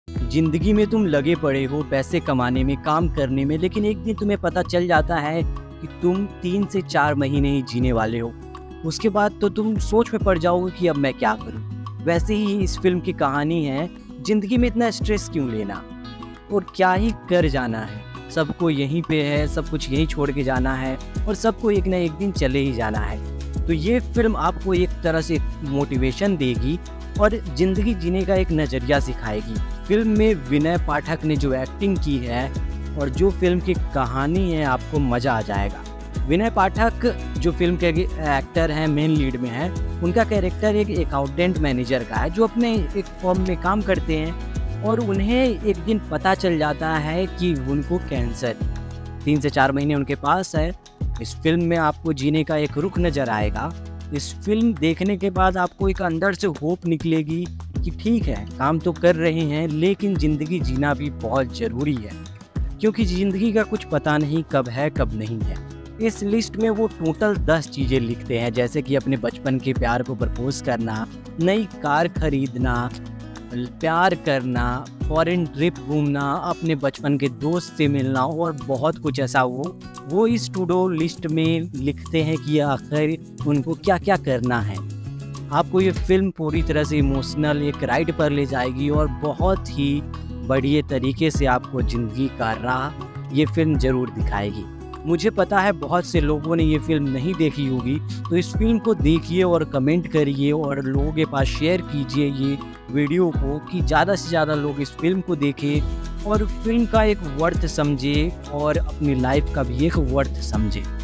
Dasvidaniya Movie Review | Motivational Movie | Inspirational Movie
In this video i am talking about best inspirational & motivational movie